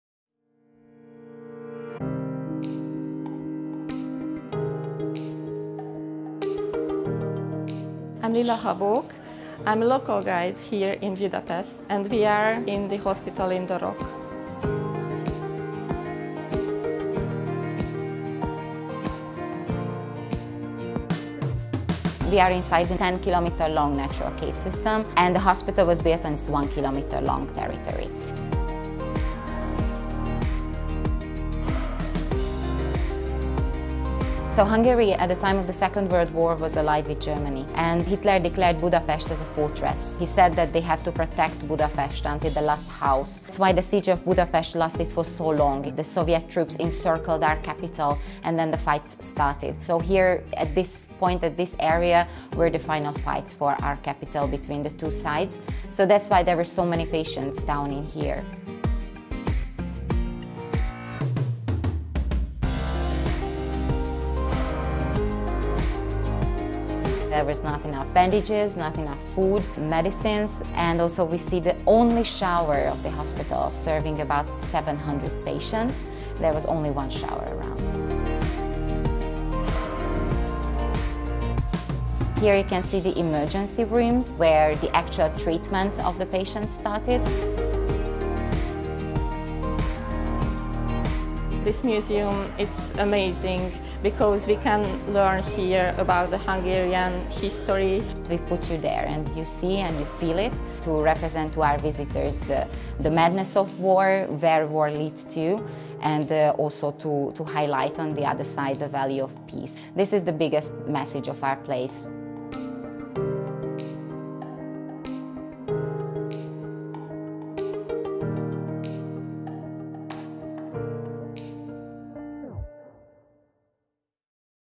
Exploring Budapest’s Hospital in the Rock - Local Favorites
Local Guides like Lilla love sharing historic sites in their cities. Watch as she tours a local favorite in Budapest, Hungary, the Hospital in the Rock. This hospital and bomb shelter was built during World War II into a 1 kilometer stretch of a 10 kilometer natural cave system underneath Buda Castle.

     282018 Tracker
 YouTube Footer and Series Guidelines
 16[2017] Current
 13[2017] Posted Videos

Explore